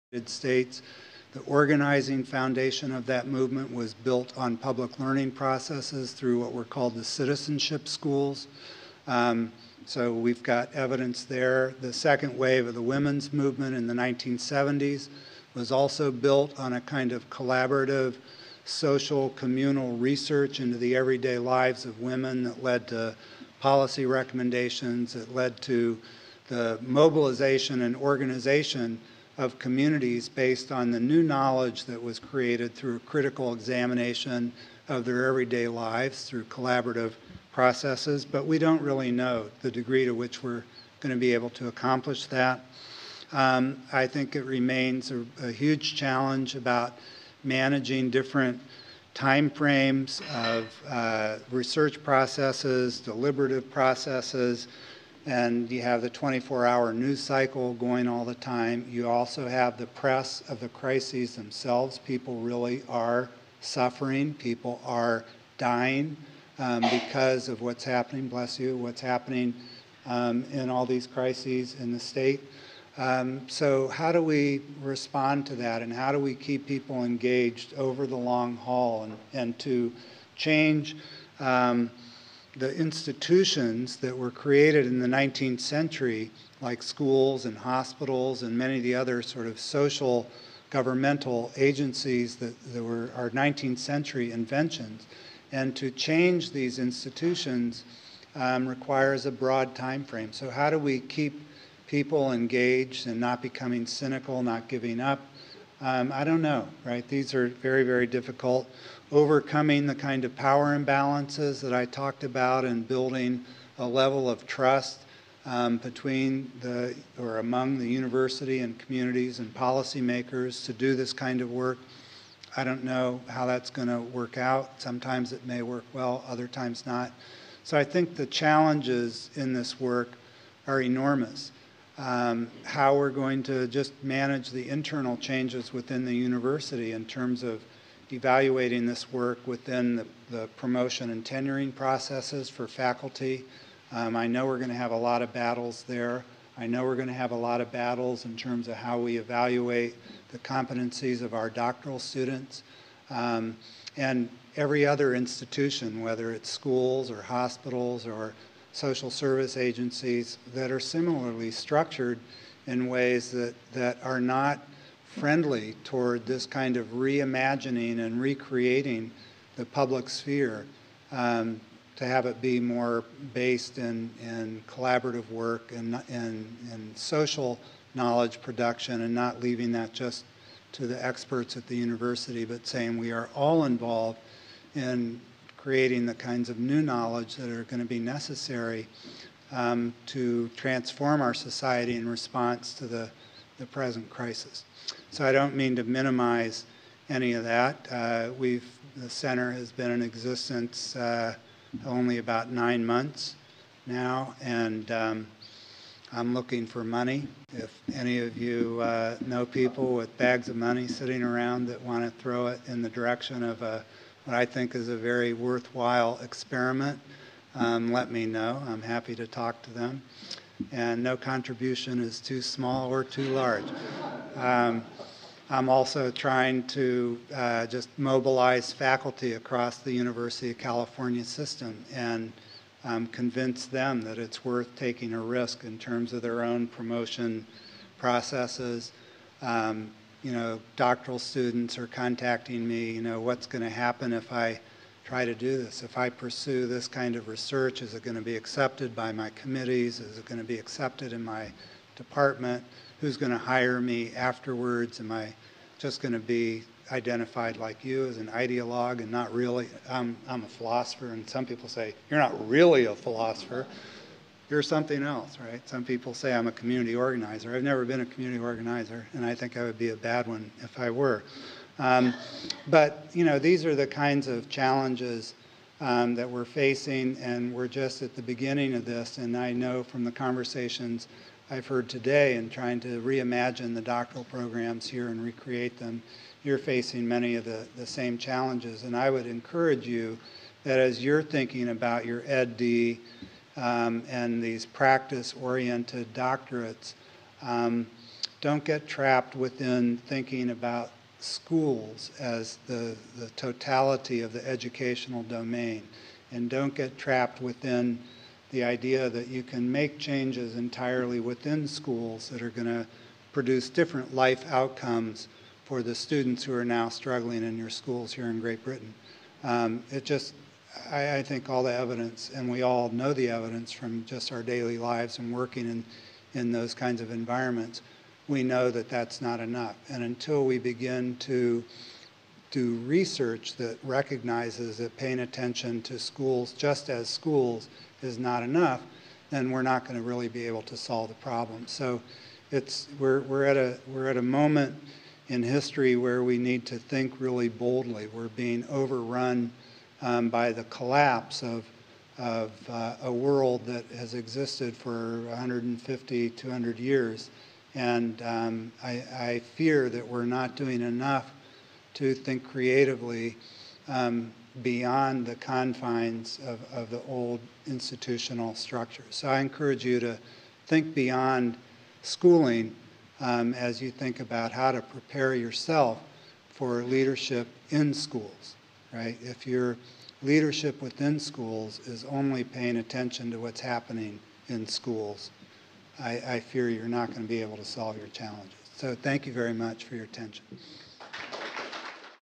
Prof Ronald Glass Part 6
Prof Ronald Glass Key Note Part 6 BELMAS DRIG Seminar Series, Second Seminar, Institute of Education, London, 28th March 2011.